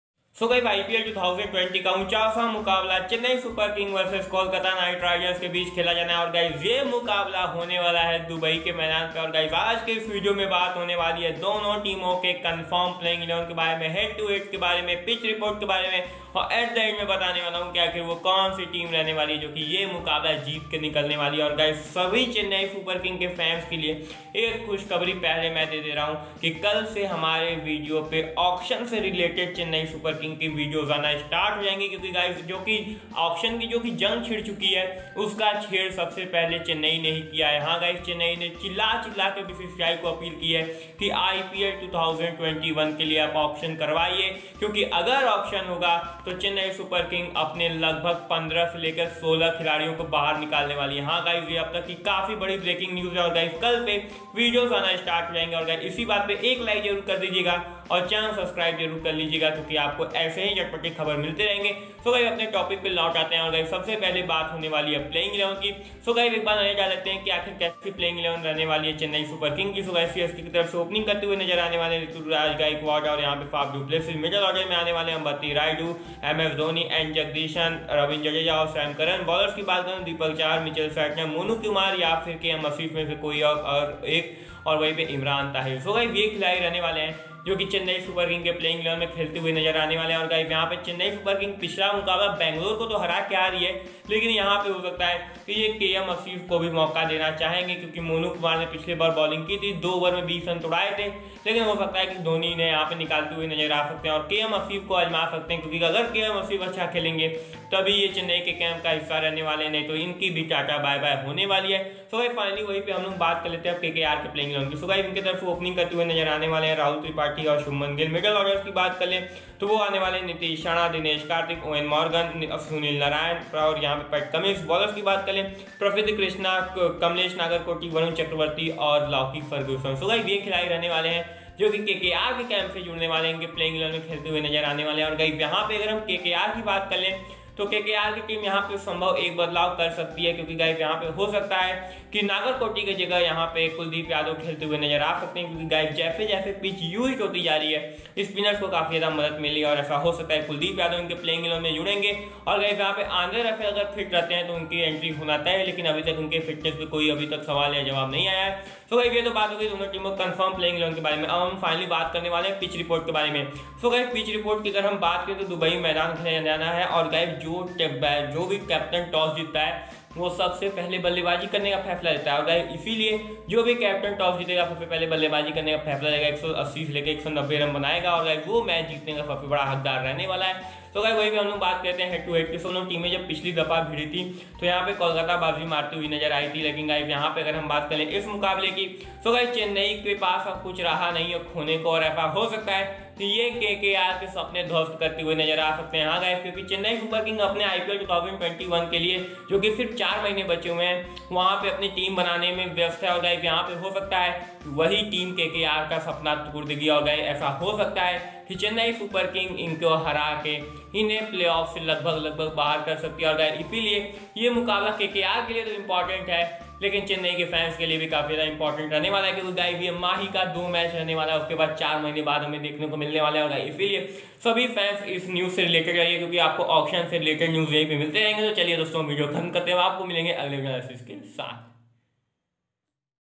IPL 2020 - Chennai Super Kings vs KolkataKnight Riders Preview - 29 October 2020 | Dubai
WatchIPL 2020 - Chennai Super Kings vs KolkataKnight Riders Preview - 29 October 2020 | Dubai.